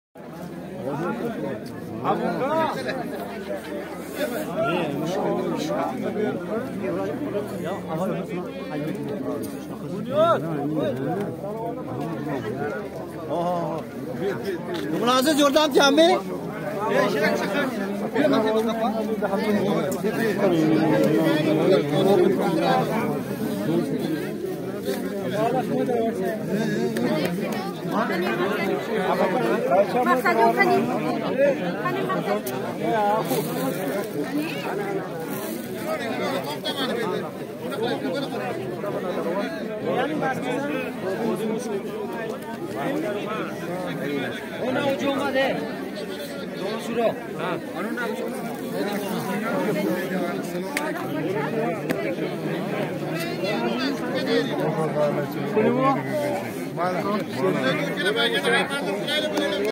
ВОДИЙ МУСЛИМ ТУР АГЕНТЛИГИ ЗИЁРАТЧИЛАРИ НАМАНГАН АЭРОПОРТИГА КИРИБ КЕЛДИЛАР
КУЧОКЛАШИБ КУРИБ САФАРГА ЖУНАТИШ - ОТА БОБОЛАРИМИЗДАН КОЛГАН МЕРОС